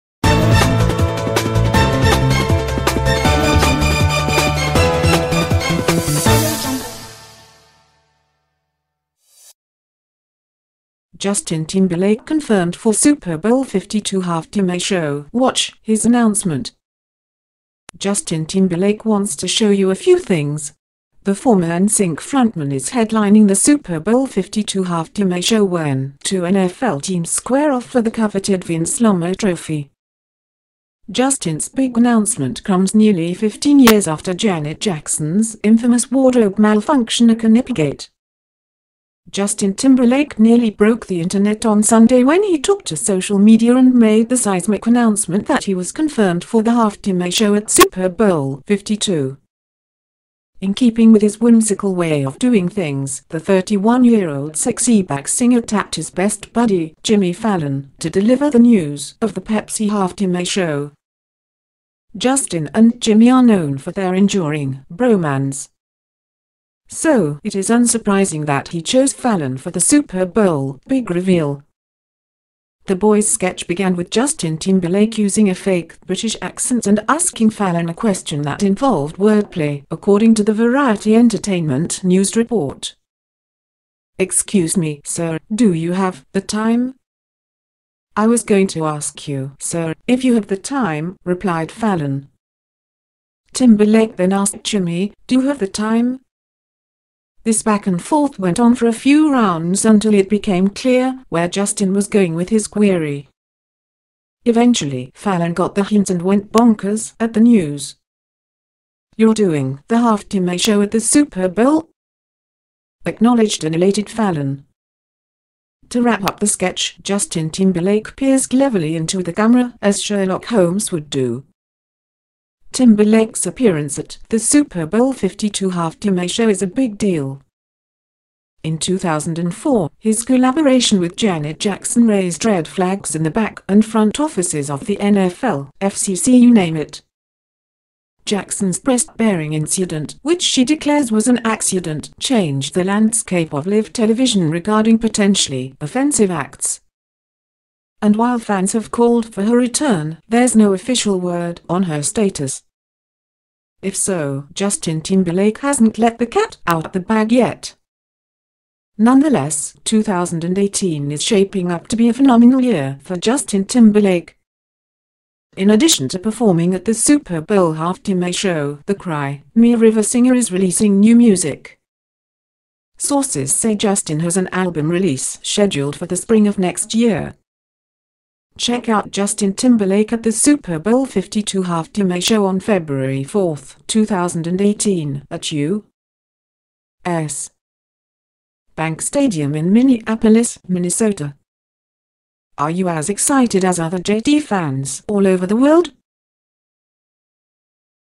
JUSTIN TIMBERLAKE CONFIRMED FOR SUPER BOWL 52 HALFTIME SHOW, WATCH HIS ANNOUNCEMENT.
Justin Timberlake wants to show you a few things: The former ‘NSYNC frontman is headlining the Super Bowl LII Halftime Show when two NFL teams square off for the coveted Vince Lombardi Trophy. Justin’s big announcement comes nearly 15 years after Janet Jackson’s infamous wardrobe malfunction aka “Nipplegate.”